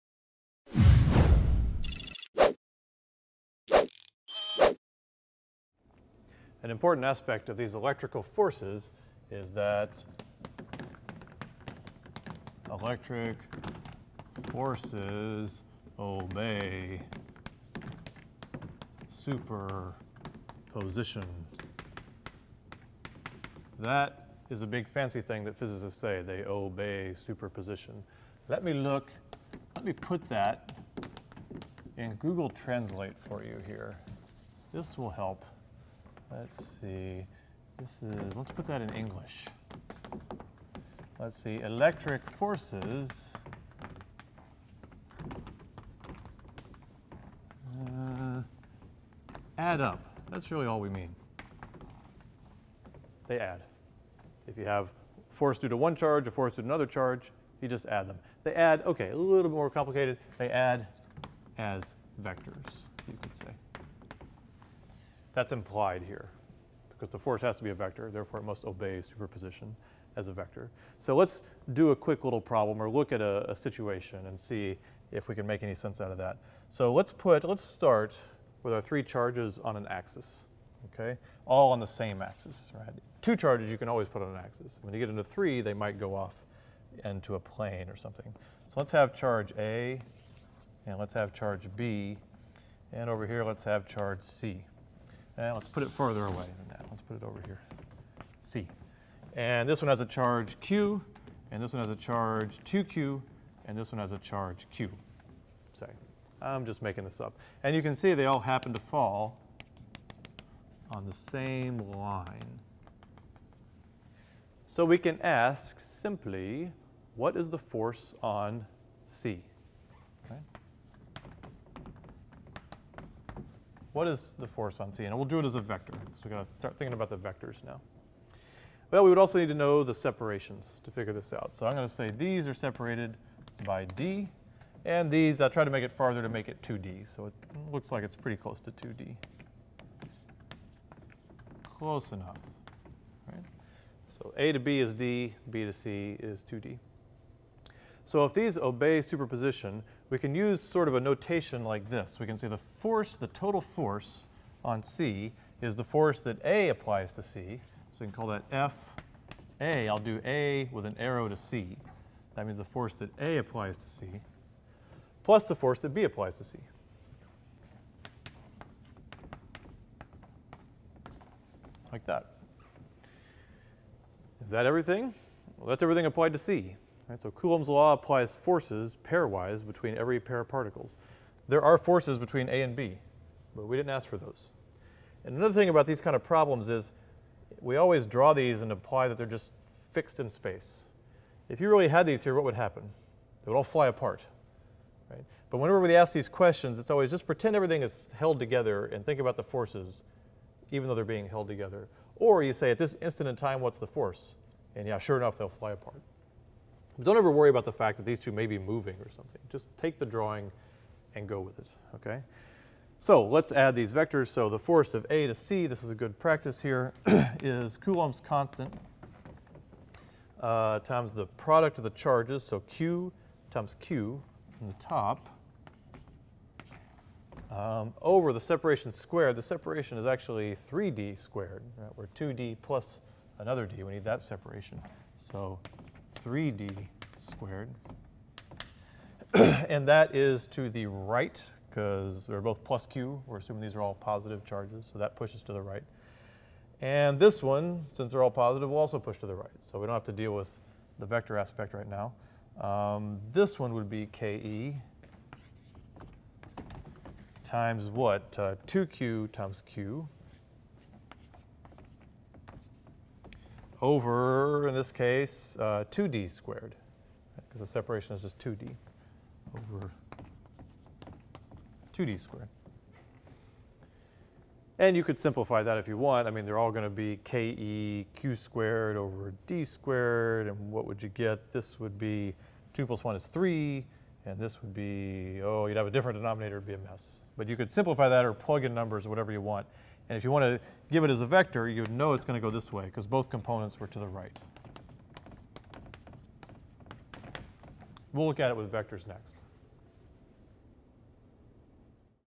OpenStax AP Physics Chapter 18: Superposition of Charges in 1D
This instructional video covers Electric Charge and Electric Field and corresponds to Chapter 18 in OpenStax College Physics for AP® Courses.